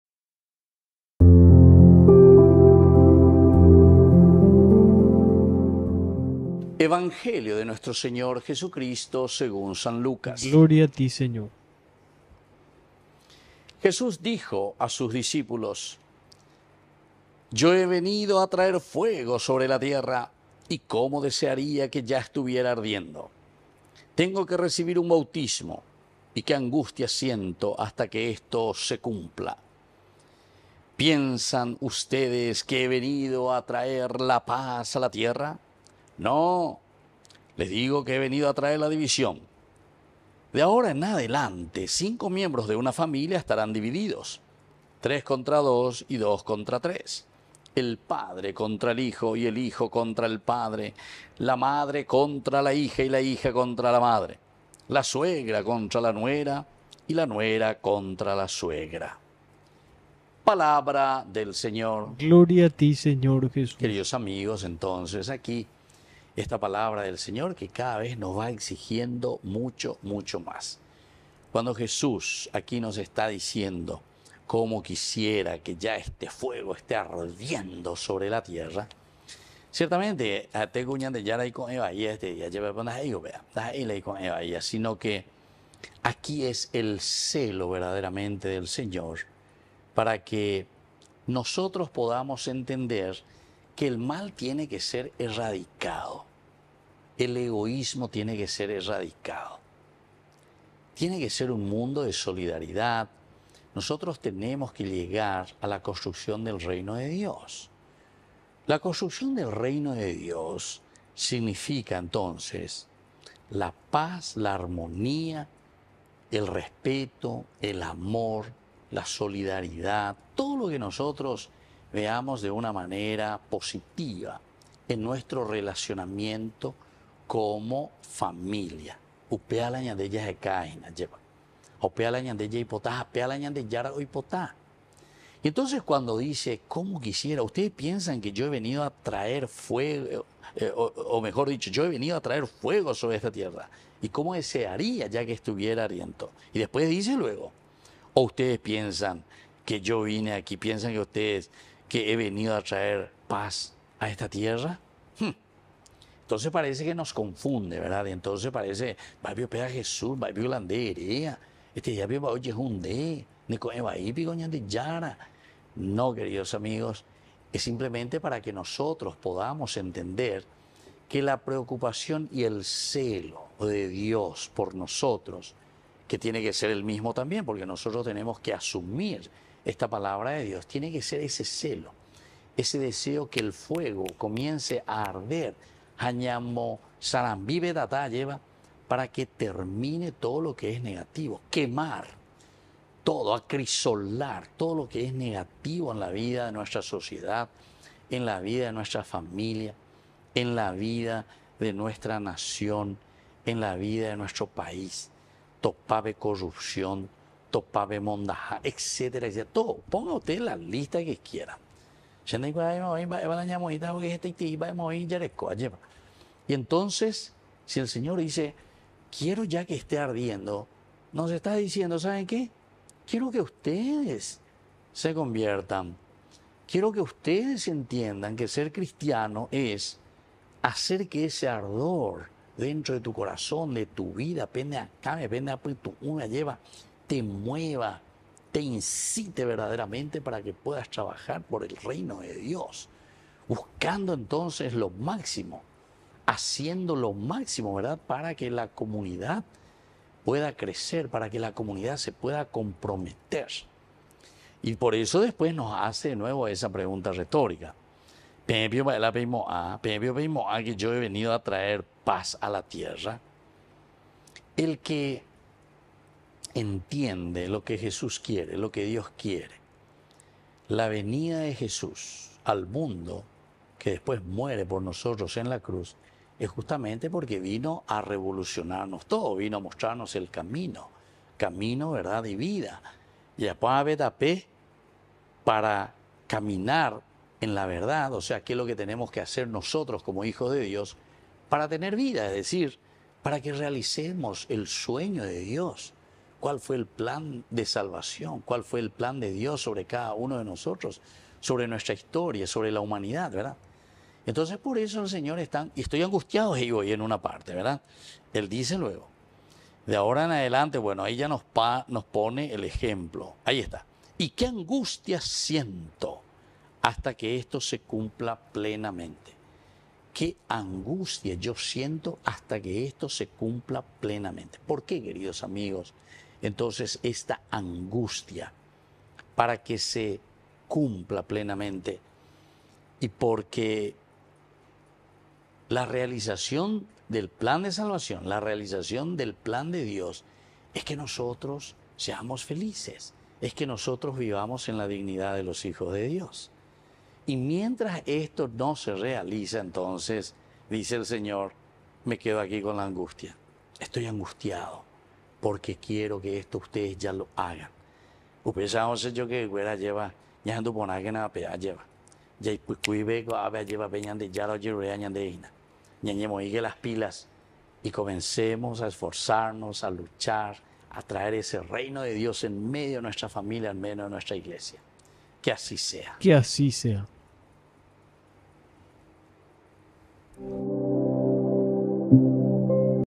HOMILIA DE HOY | Jueves 26 de octubre del 2023
Santo Evangelio según San Lucas 12, 49-53:
Jesús dijo a sus discípulos: Yo he venido a traer fuego sobre la tierra, ¡y cómo desearía que ya estuviera ardiendo!